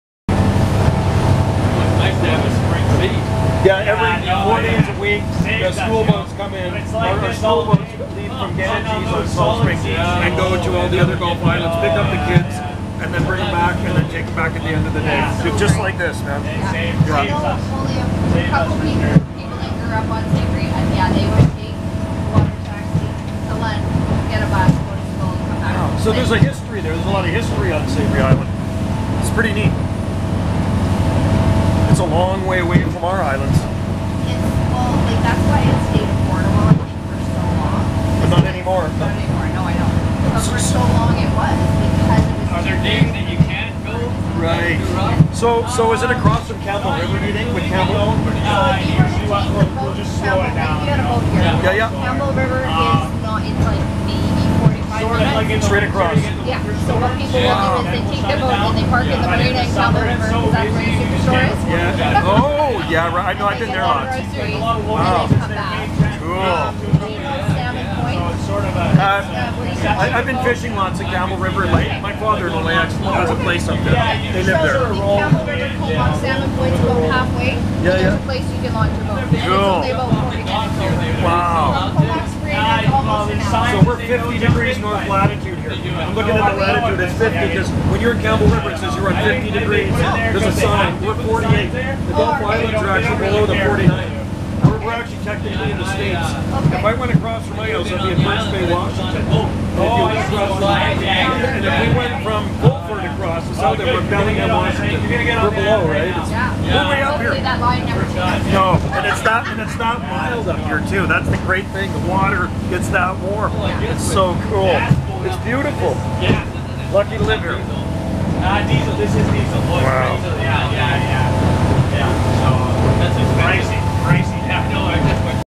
Water Taxi from Lund to Savary Island
Quick boat ride from Lund on the Sunshine Coast to Savary Island.